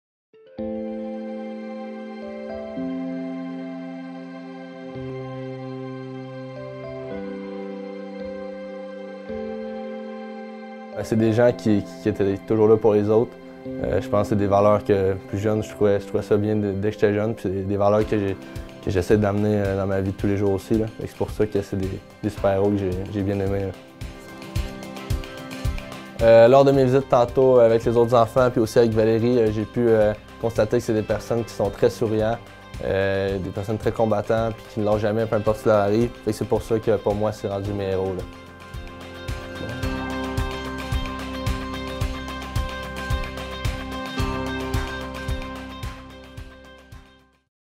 Mois des petits héros avec Nathan Ouellet
Nathan Ouellet a été impressionné par Valayrie. Malgré qu’elle soit atteinte du spina-bifida, elle demeure toujours positive.Tout le mois de février, au nom des enfants, soyez généreux